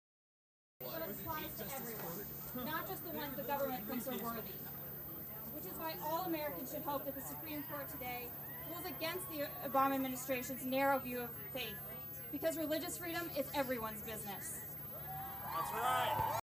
'Religious freedom is everyone's business'
"Religious freedom is everyone's business," says an activist outside the Supreme Court March 25 as the justices were hearing oral arguments on the healthcare law's contraceptive services mandate.